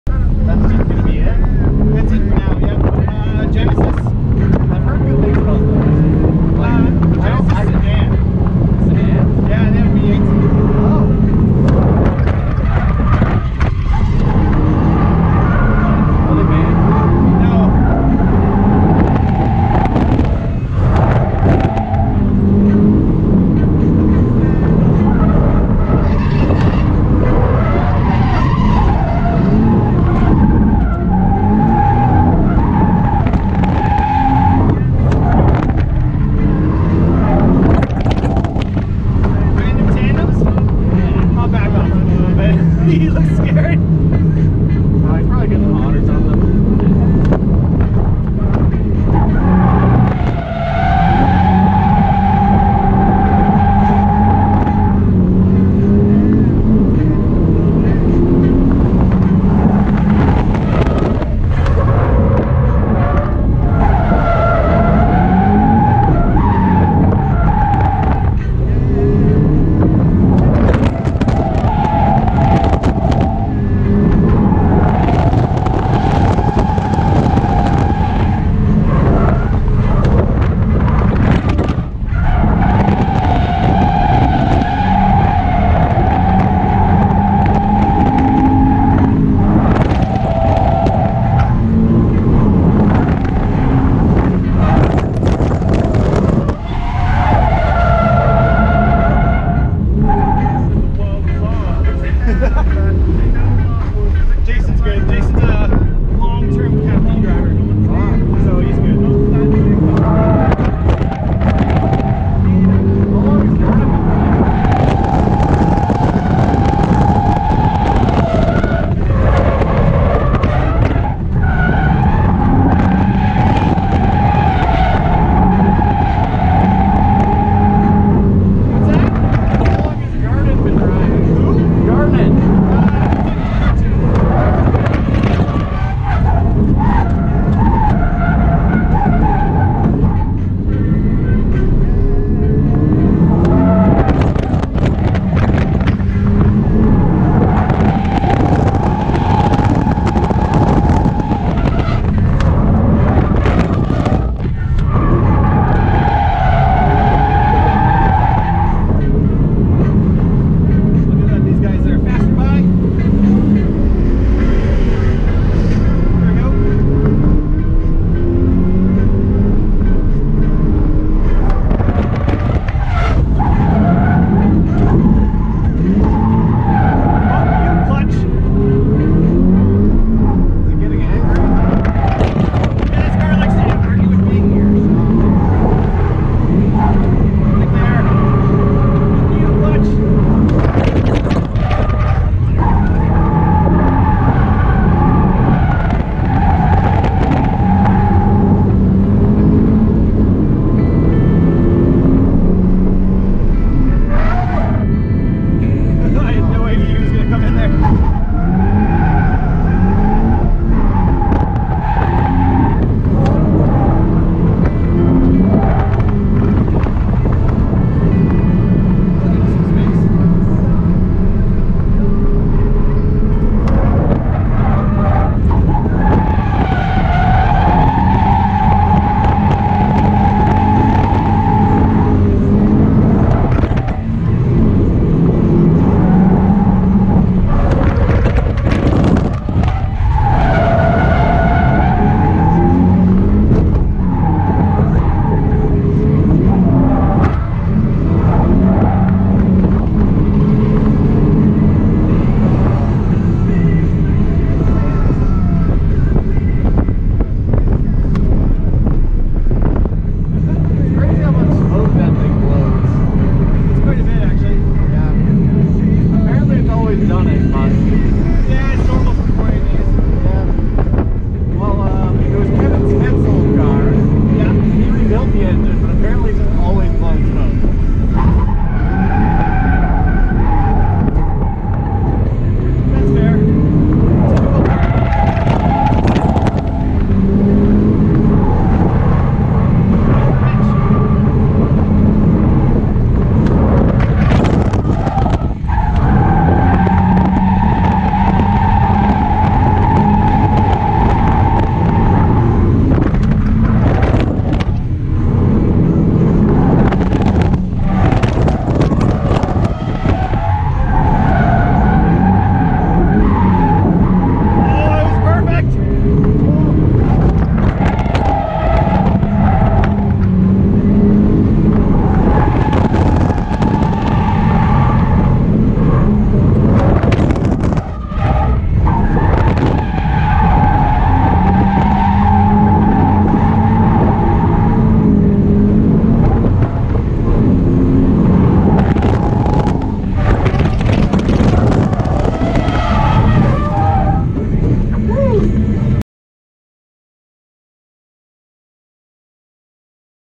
Lil crunch raw
Having some fun at an open track day. Got a little too jimmy on the shimmy.